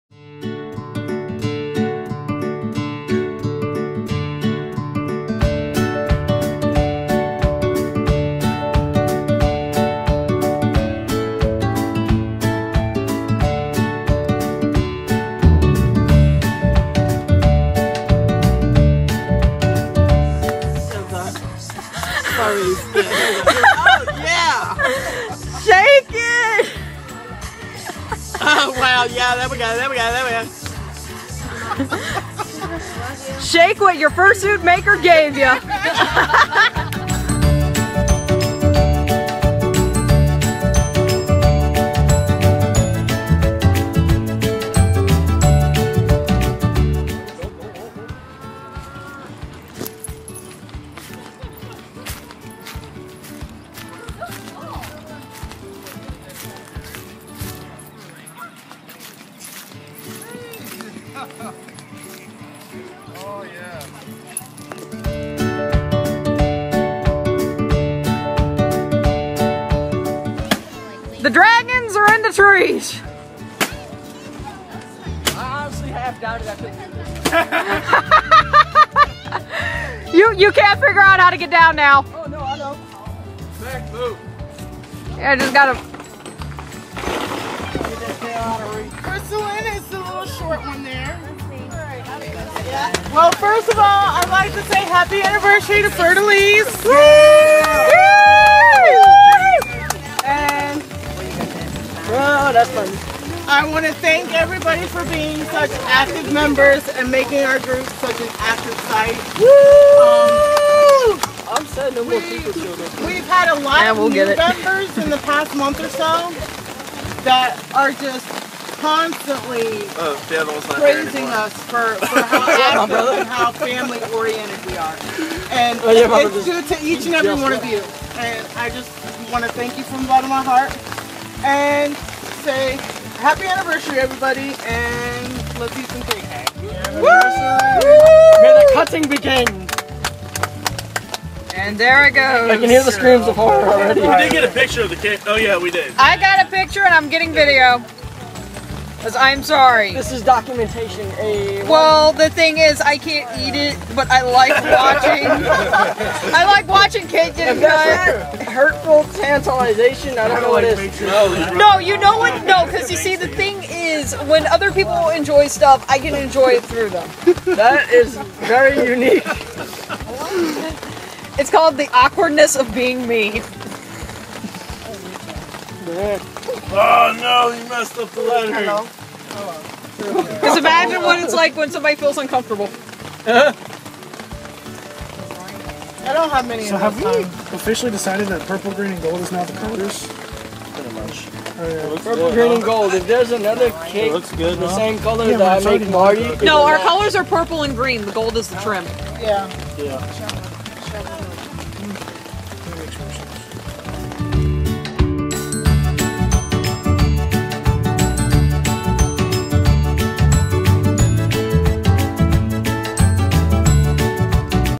first fursuit meet with the Louisiana Fur De Lis
this my first meet up with my fursuit group im my state. many of us come from many walks of life and we are starting a antibully program this was the one year celebration of the group being formed and to tell the truth though my head was not done I still had fun